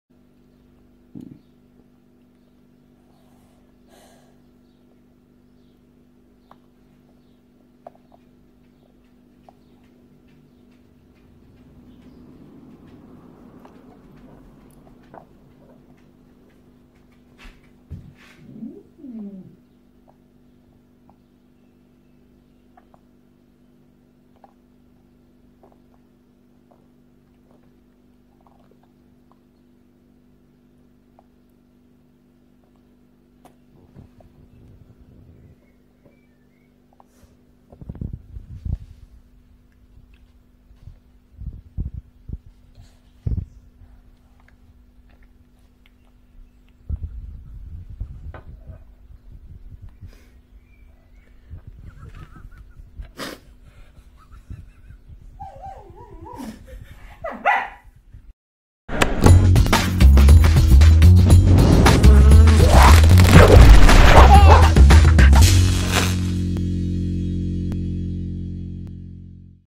Baby literally falls asleep while eating snack
Watch this 1-year-old boy call it quits after trying to stay awake for some food. Cuteness overload!